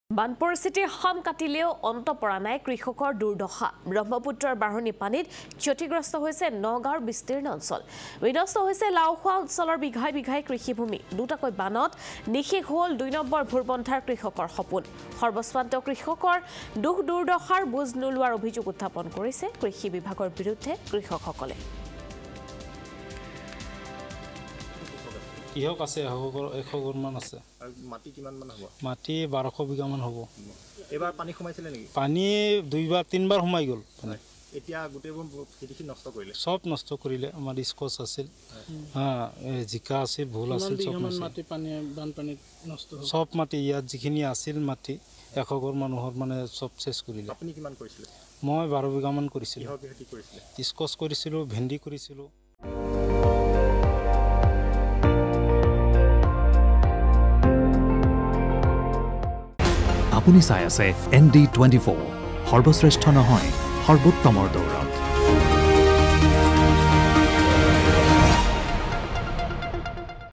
🔲 ব্ৰহ্মপুত্ৰৰ বাঢ়নী পানীত ক্ষতিগ্ৰস্ত নগাঁৱৰ বিস্তীৰ্ণ অঞ্চল
🔲 বান পৰিস্থিতি শাম কাটিলেও অন্ত পৰা নাই কৃষকৰ দুৰ্দশা